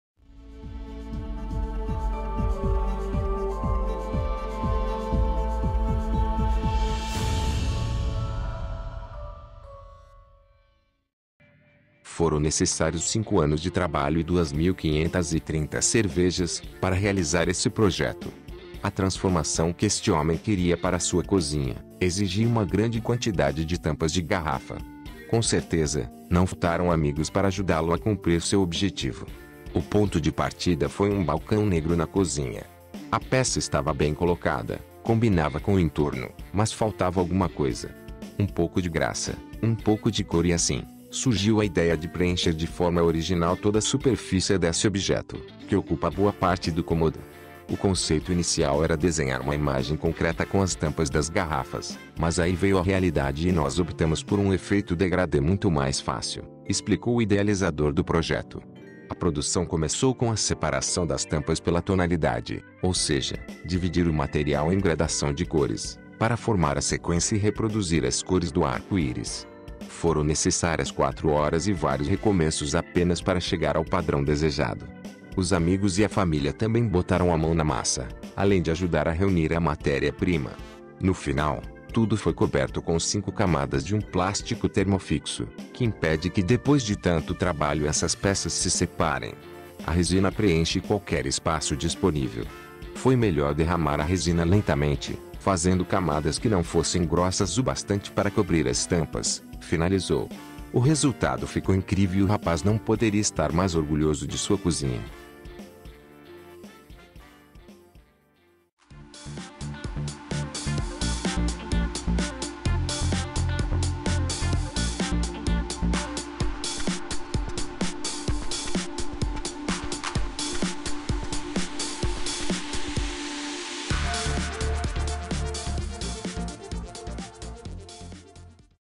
O homem que cobriu a superfície do balcão da cozinha, com tampas de Garrafas
Foram necessários 5 anos de trabalho e duas mil quinhentas e trinta cervejas, para realizar esse projeto. A transformação que este homem queria para sua cozinha, exigia uma grande quantidade de tampas de garrafa. Com certeza, não faltaram amigos para ajudá-lo a cumprir seu objetivo. O ponto de partida foi um balcão negro na cozinha. A peça estava bem colocada, combinava com o entorno, mas faltava alguma coisa. Um pouco de graça, um pouco de cor e assim, surgiu a ideia de preencher de forma original toda superfície desse objeto, que ocupa boa parte do cômodo. “O conceito inicial era desenhar uma imagem concreta com as tampas das garrafas, mas aí veio a realidade e nós optamos por um efeito degradê muito mais fácil”, explicou o idealizador do projeto. A produção começou com a separação das tampas pela tonalidade, ou seja, dividir o material em gradação de cores, para formar a sequência e reproduzir as cores do arco-íris.  Foram necessárias 4 horas e vários recomeços apenas para chegar ao padrão desejado. Os amigos e a família também botaram a mão na massa, além de ajudar a reunir a matéria prima. No final, tudo foi coberto com 5 camadas de um plástico termofixo, que impede que depois de tanto trabalho essas peças se separem. “A resina preenche qualquer espaço disponível. Foi melhor derramar a resina lentamente, fazendo camadas que não fossem grossas o bastante para cobrir as tampas “, finalizou. O resultado ficou incrível e o rapaz não poderia estar mais orgulhoso de sua cozinha!